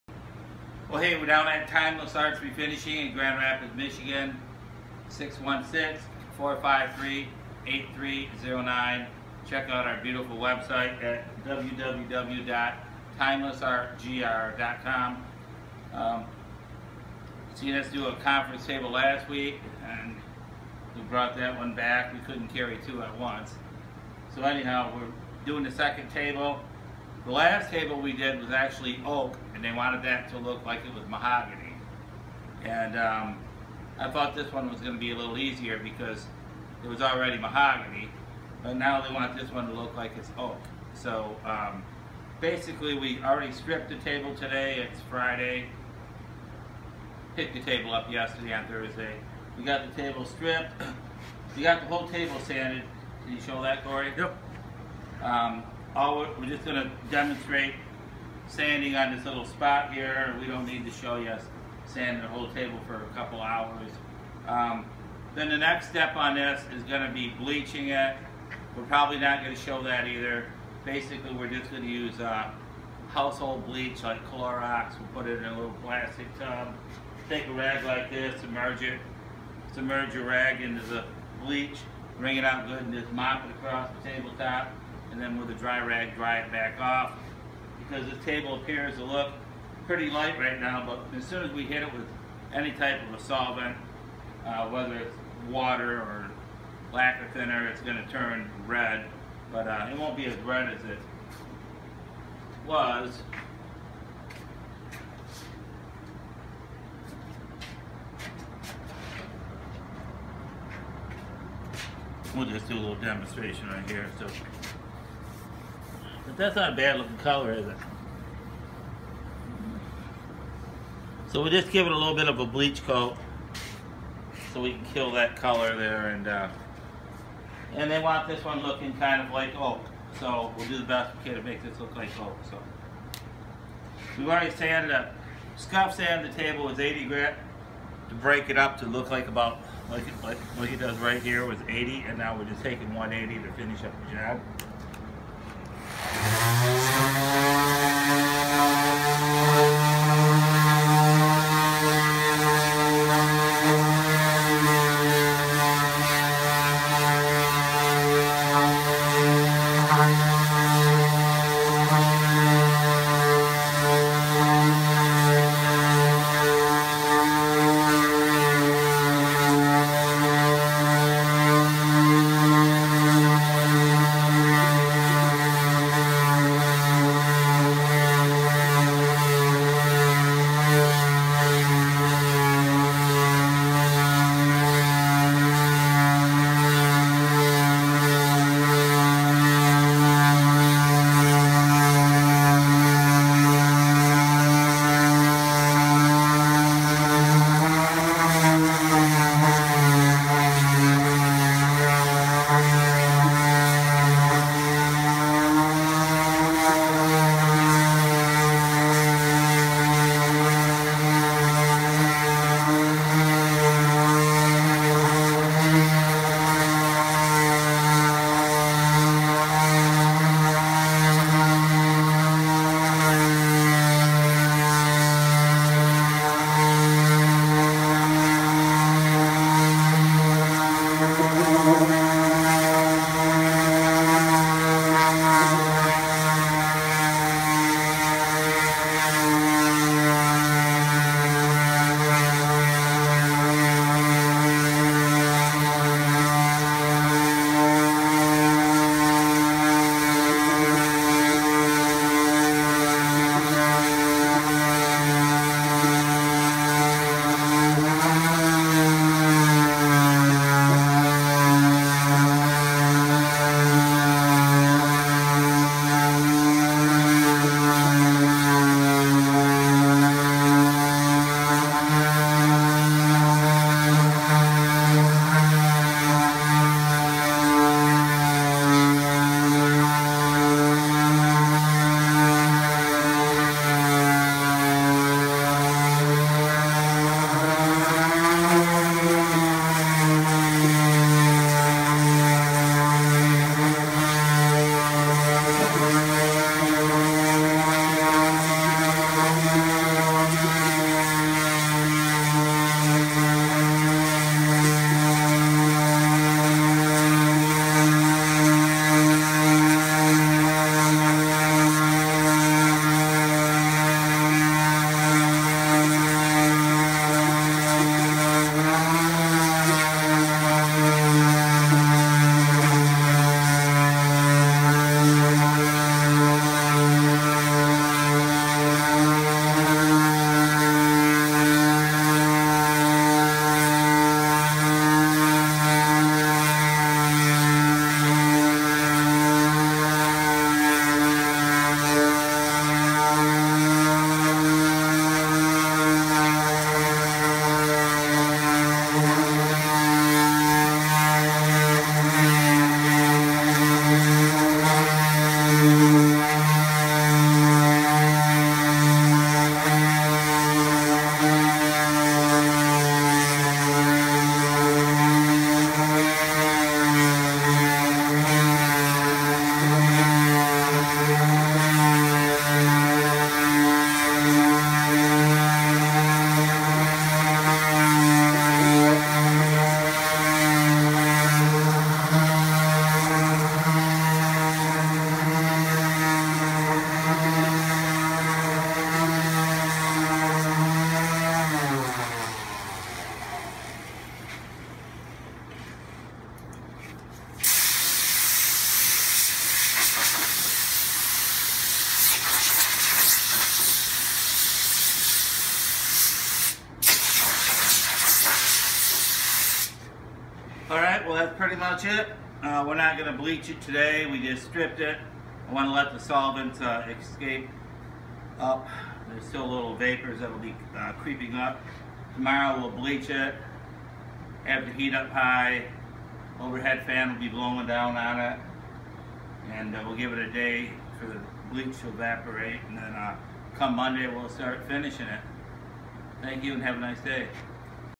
refinishing a mahogany conference
timeless arts refinishing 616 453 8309 show,s a mahogany conference table that we are refinishing for a office down town grand rapids mi.this table will be but into a oak finish .